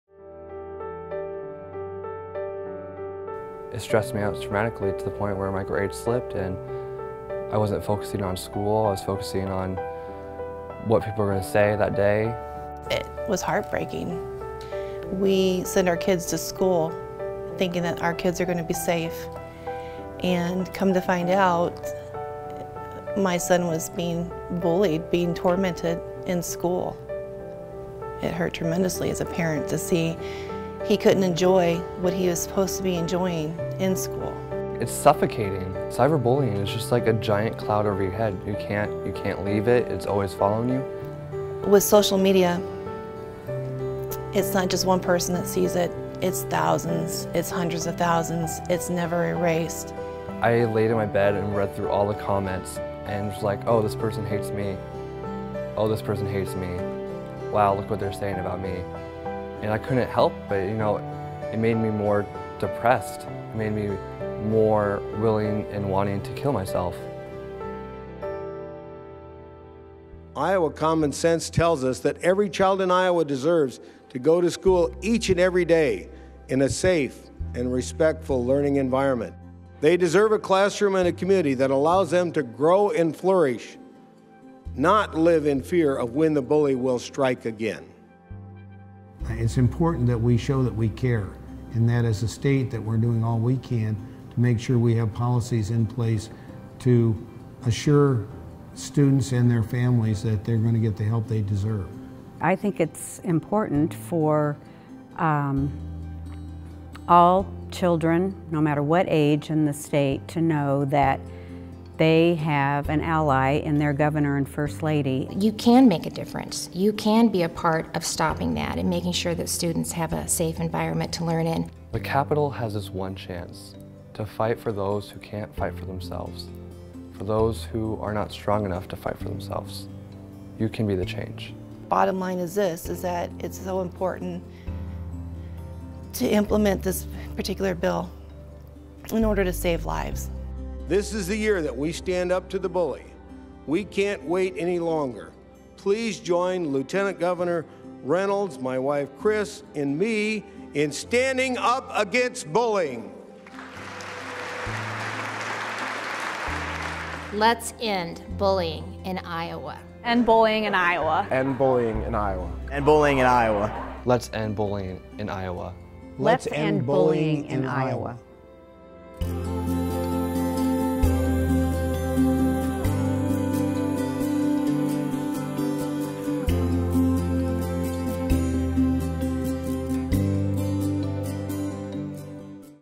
Let's End Bullying in Iowa!
Gov. Branstad, First Lady Chris Branstad and Lt. Gov. Reynolds call for the passage of the Bully-Free Iowa Act of 2015. Join the conversation on Facebook and Twitter by using the hash tag EndBullyingIA.

With thanks for The University of Northern Iowa Foundation and Verizon.